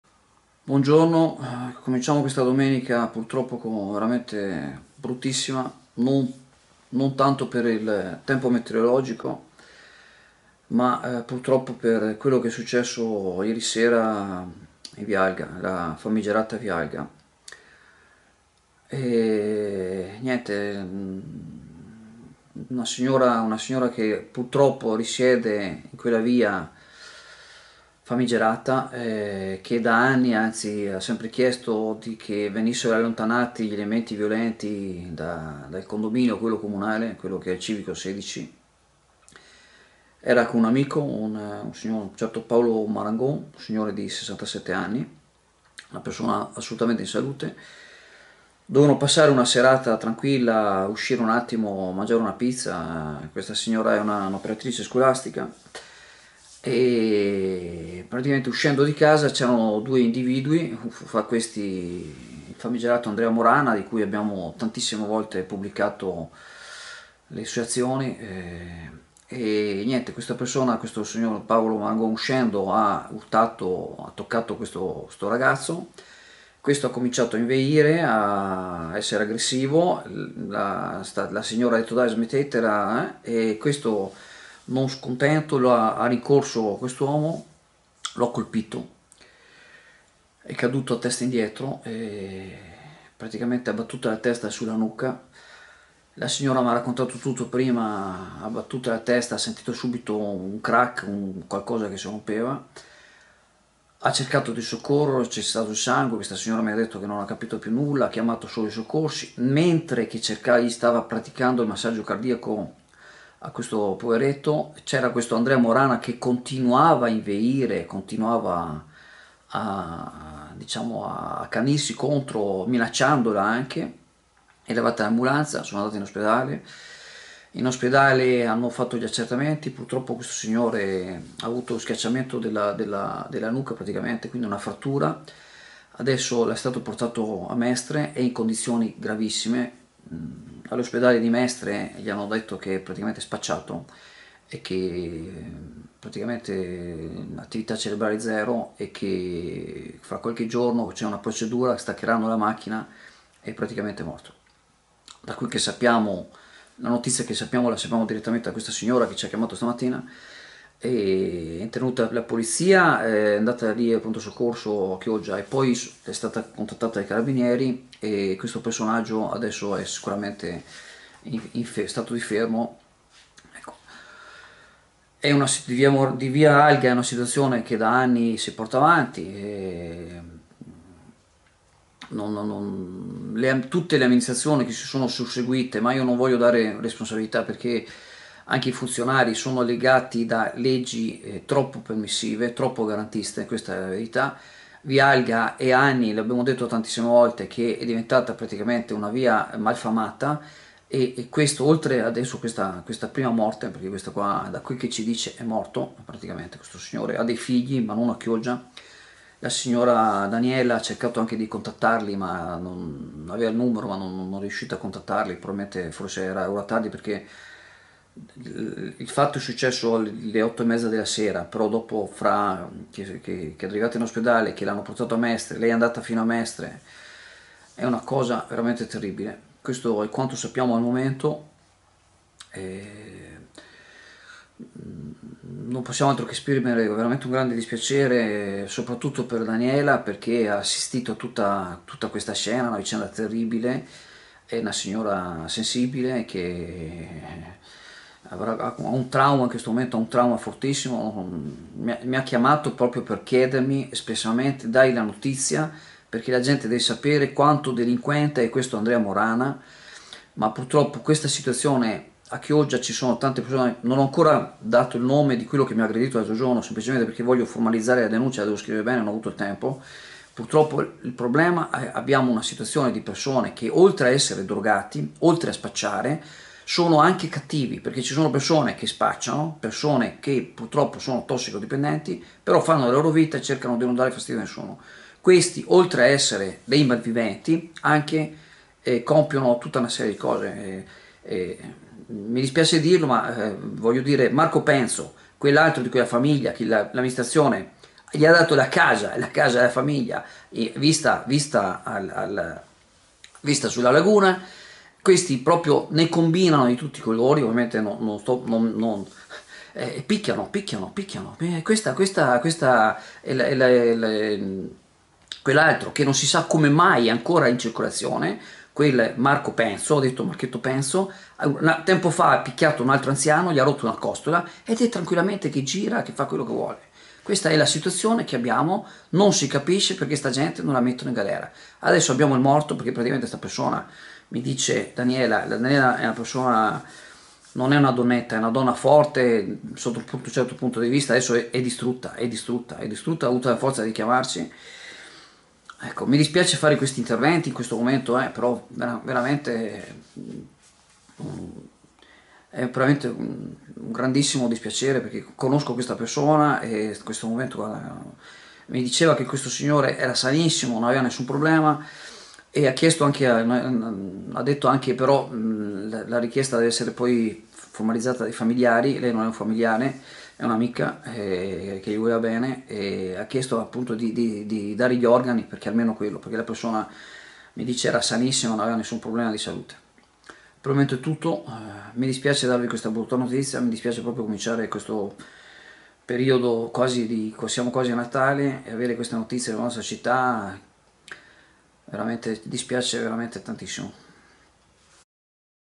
AGGREDITO IN VIA ALGA DAL PREGIUDICATO ANDREA MORANA: È IN STATO DI MORTE CEREBRALE A MESTRE
Ieri sera attorno alle 20.30 quella che doveva essere una tranquilla serata di due amici, lei un’operatrice scolastica lui un pensionato di 67 anni è diventata purtroppo una tragedia, l’uomo Paolo Marangon si trova da ieri a Mestre, intubato, in stato di morte cerebrale.
Ci ha chiamato la signora Daniela, sconvolta ci ha supplicato di dare la notizia, di dire a tutti quello che è accaduto.
In estrema sintesi il 67enne è stato aggredito dal pregiudicato Andrea Morana per futilissimi motivi, lo ha colpito:”È caduto all’indietro come un birillo - ci riferisce la povera Daniela - ho sentito un rumore di qualche cosa che si rompeva” . Da lì i soccorsi, mentre il Morana continuava ad inveire e a minacciare di picchiare anche lei mentre tentava di praticargli la rianimazione cardiaca. Al pronto soccorso a Chioggia non hanno potuto far altro che constatare la gravità del trauma e portarlo di corsa all’ospedale dell’Angelo. “I medici di Mestre mi hanno già detto, non c’è nulla da fare, si tratta di morte cerebrale”.
“Ho cercato do contattare i figli ma non mi hanno risposto, li ho chiamati nel cuore della notte”. Si spera nella donazione degli organi del poveretto che “era perfettamente sano”.
Oltre al primo intervento della polizia c’è stato il successivo intervento dei carabinieri il pregiudicato Andrea Morana dovrebbe essere agli arresti.